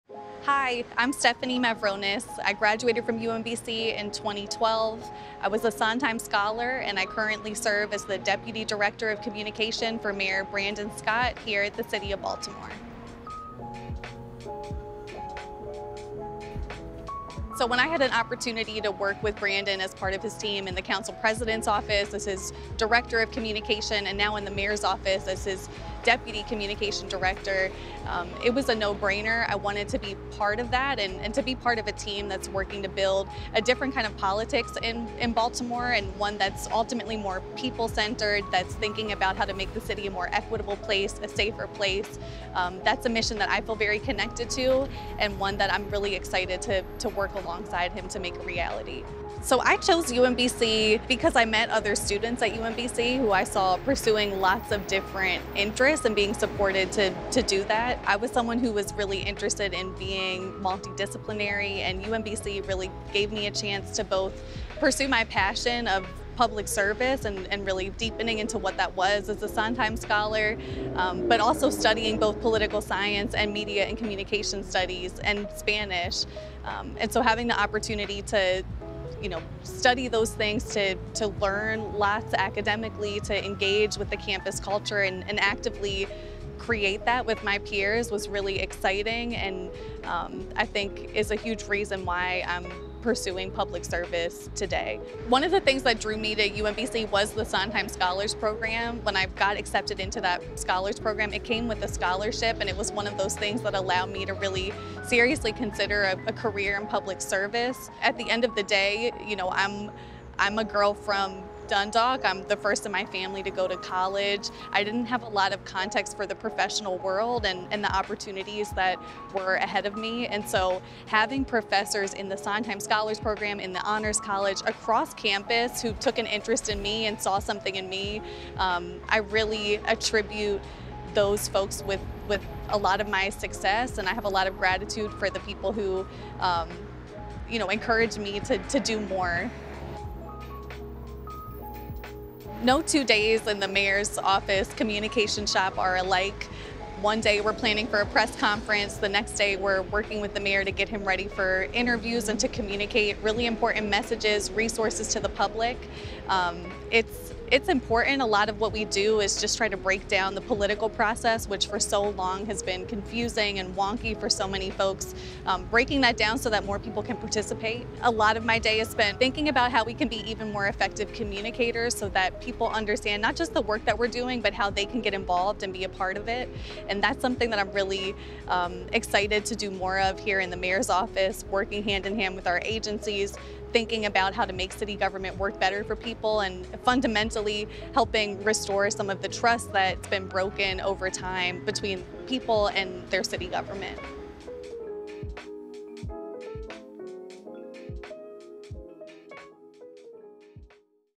Building a Better Baltimore, Together — Stefanie Mavronis '12
As a first-generation college student from southeast Baltimore, Sondheim Scholar Stefanie Mavronis ’12, political science and media & communication studies, was initially hesitant to talk openly about her past. As she made a home—and a name for herself as an organizer—on campus, Mavronis realized that she could bring her whole self to UMBC. Now deputy director of communication for Baltimore City Mayor Brandon Scott, Mavronis channels her hometown pride and Retriever confidence to tackle connecting with constituents during a pandemic and what feels like an all-time low of public trust in government.

"I’m excited to be tasked with the opportunity and responsibility to be part of shaping this administration and bringing more people into this project of building a stronger Baltimore City," says Mavronis, "one that’s more equitable and accountable and that works better for people."